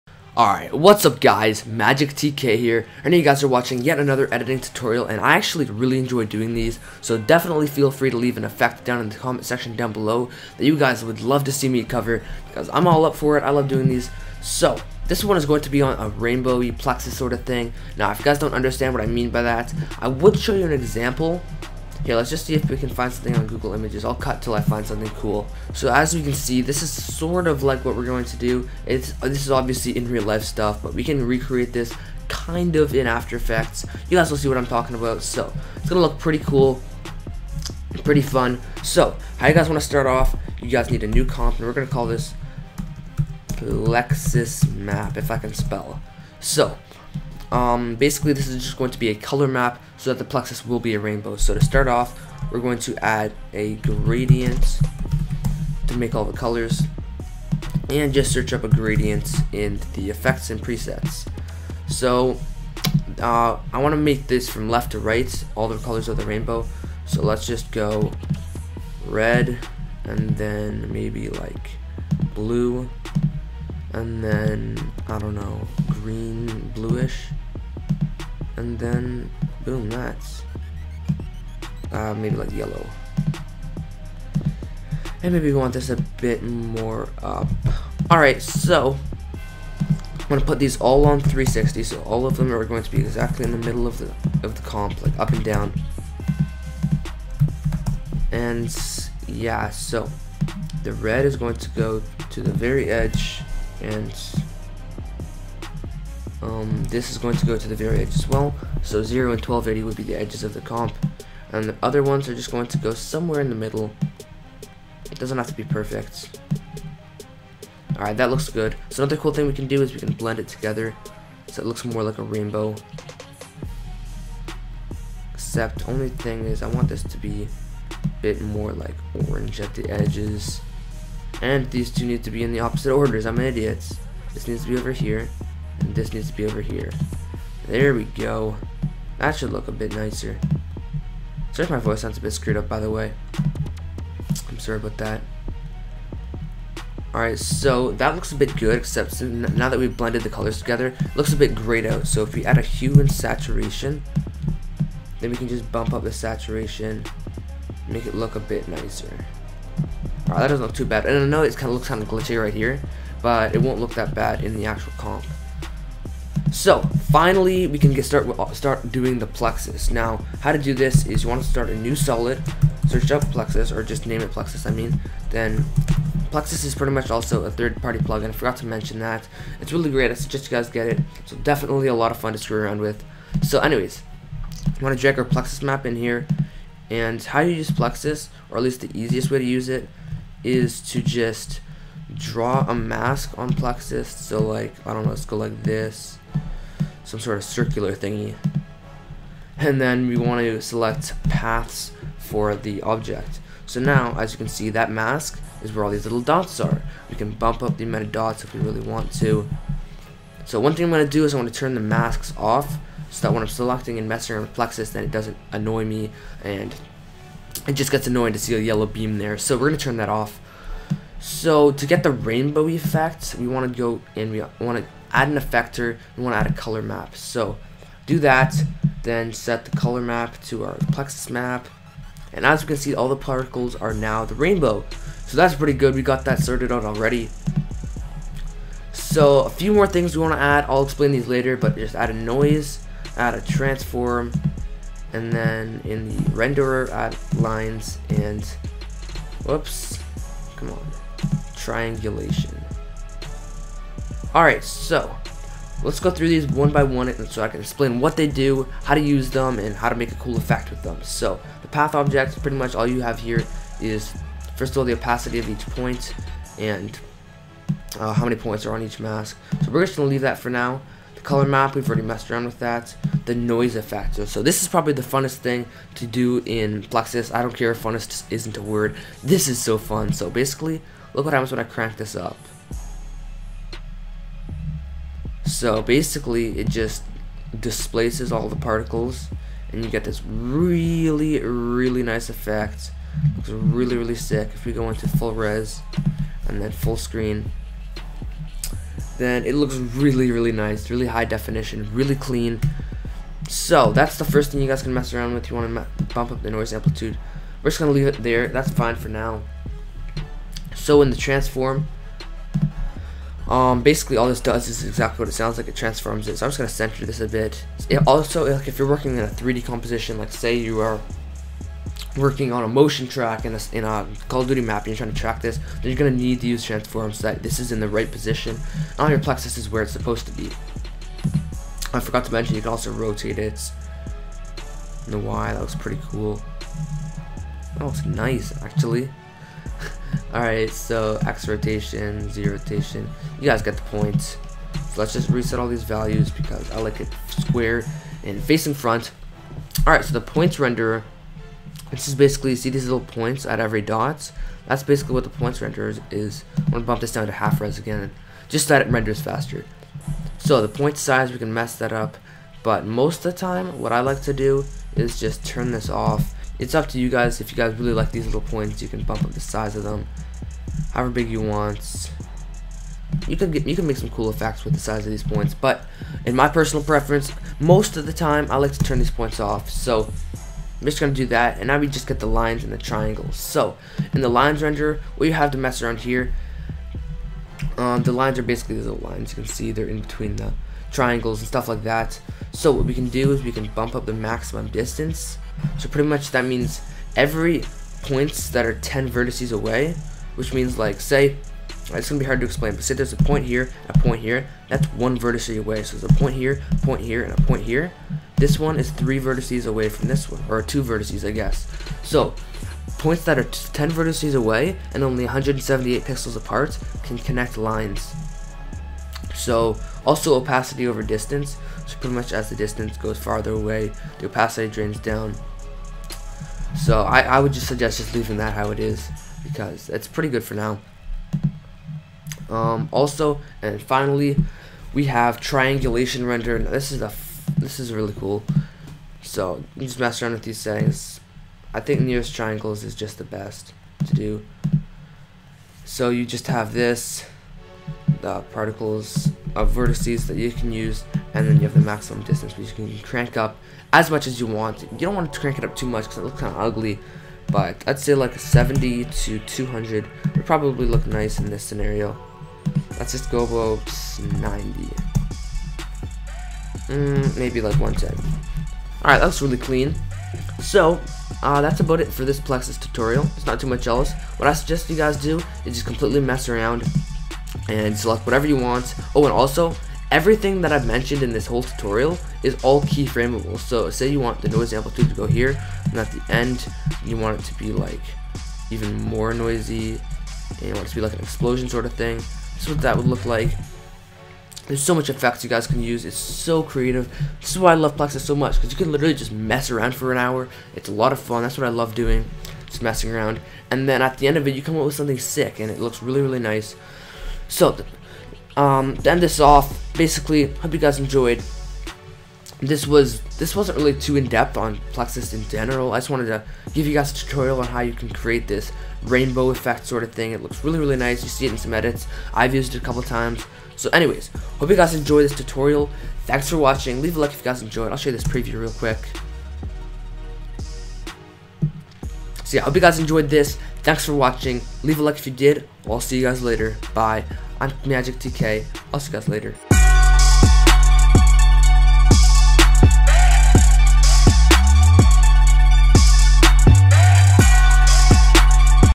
Rainbow Plexus | After Effects Tutorial!
Leave a LIKE for this sick tut!?! Please tell me what effects you want me to cover next in the tutorial!

I have permission to use SuperstaarBeat's music.